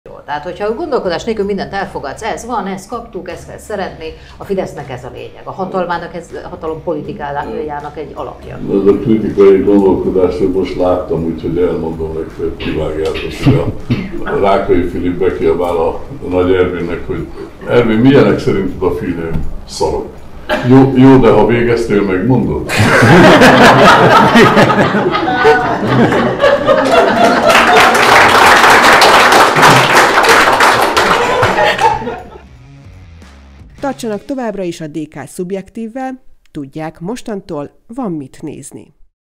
Mi a közös a kritikai gondolkodásban és Rákay filmjeiben?
Tudod mi a közös a kritikai gondolkodásban és Rákay filmjeiben? Kuncze Gábor merész viccben mondja el! A teljes adást itt tudod megnézni